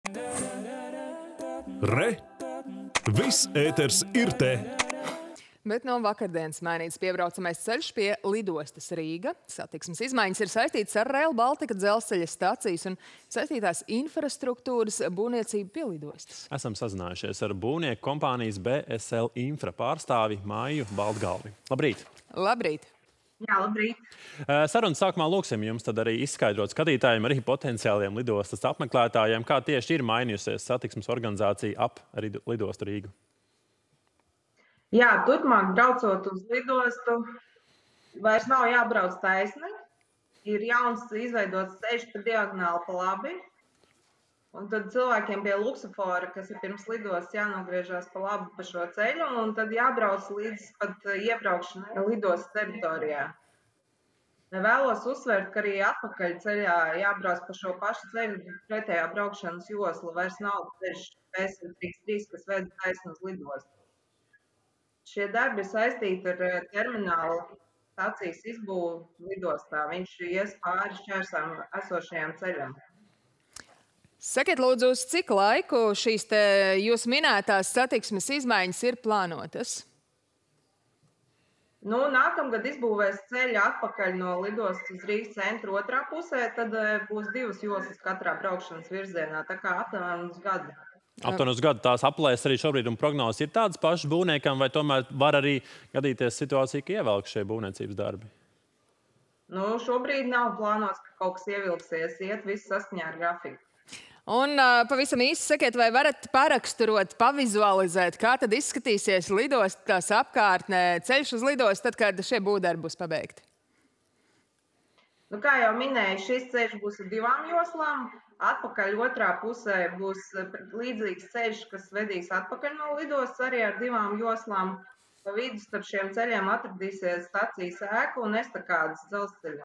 Intervija ar būvnieku apvienības “B.S.L. Infra” izpildkomitejas locekli Maiju Baltgalvi
Bet no vakardienas mainīts piebraucamais ceļš pie lidostas “Rīga”. Satikmses izmaiņas saistītas ar Rail Baltica dzelzceļa stacijas un saistītās infrastruktūras būvniecību pie lidostas.
Esam sazinājušies ar būvnieku kompānijas “B.S.L. Infra” pārstāvi Maiju Baltgalvi.